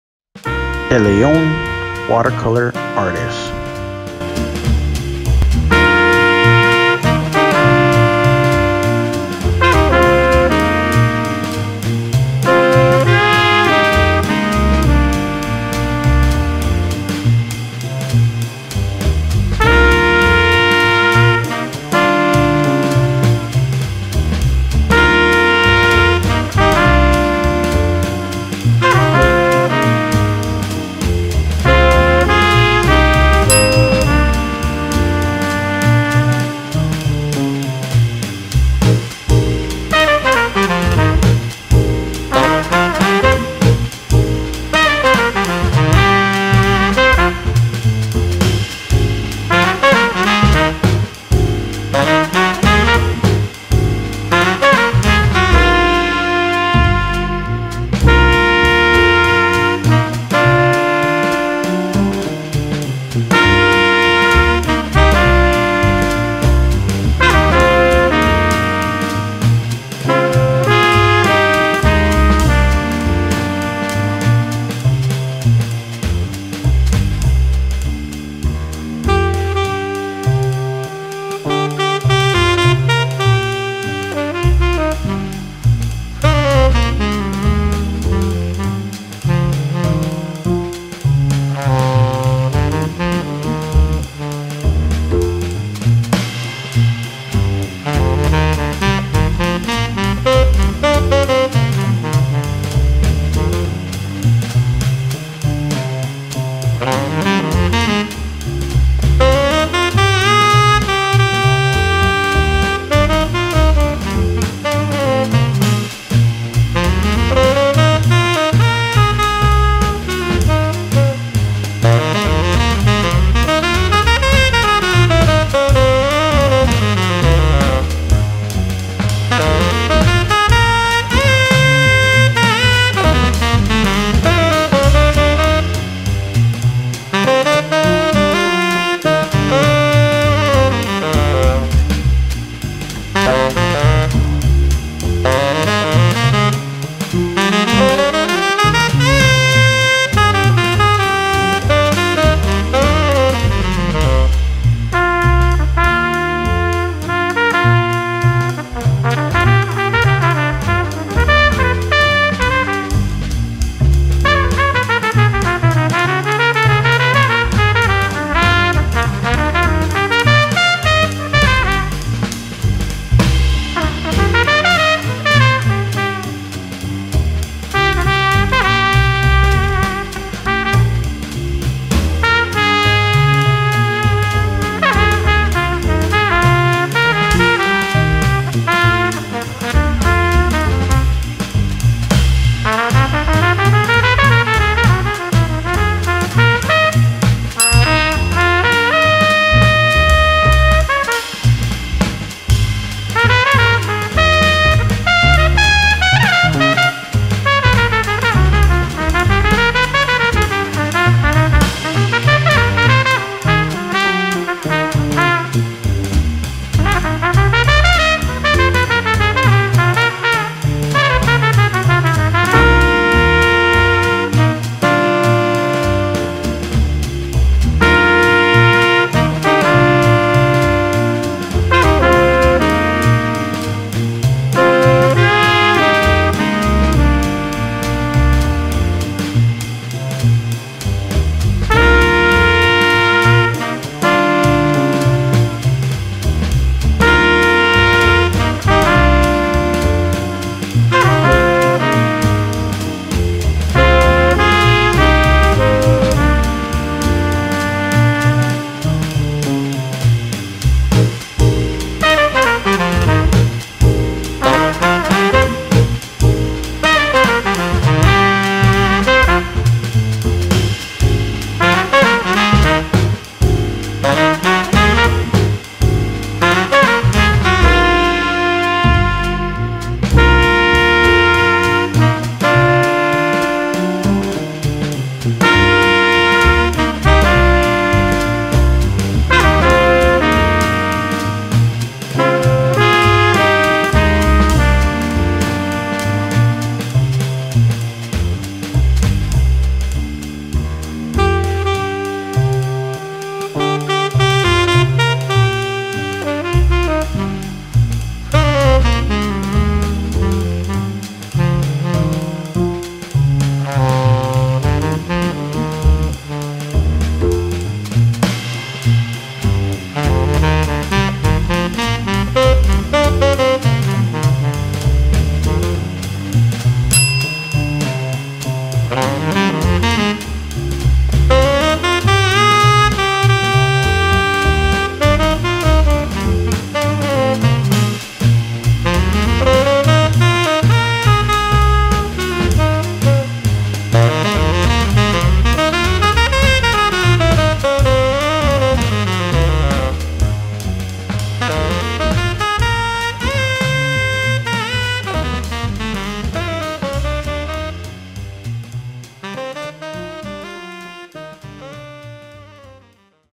How to sketch old truck in watercolor painting
Let us sketch together and get creative with watercolors.

If you like to see more watercolor, please leave a comment of what you love to see next. I would truly appreciate it a lot.

SUPPLIES I USE: (CREATE YOUR OWN AMAZING WATERCOLOR PAINTINGS)
1. Sketchbook
2. Watercolor Paint
3. Brushes
4. Pigment Liner Pens
5. Posca White Paint